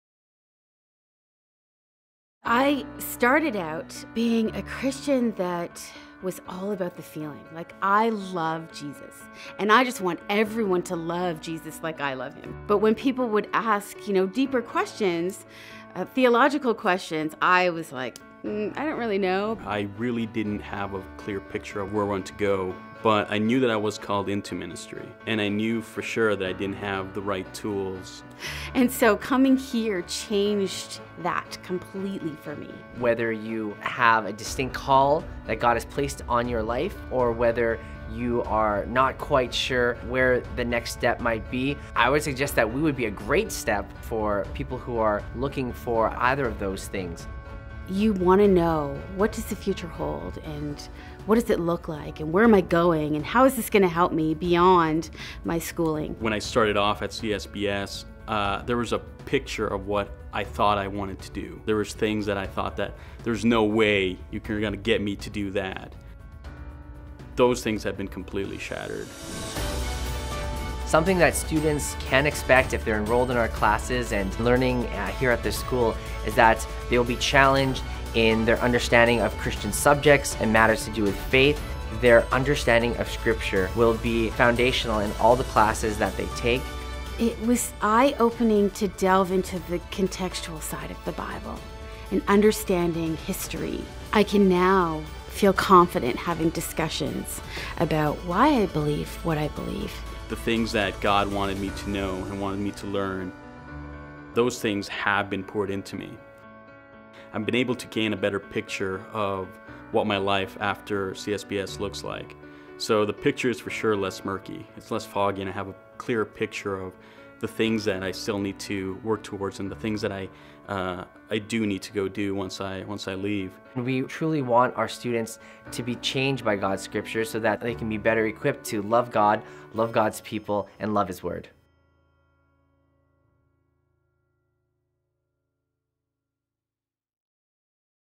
What's Your Next Step?
Whether you have a distinct calling for your life or you're not quite sure where your next step is will be, the Canadian Southern Baptist Seminary and College is a great next step.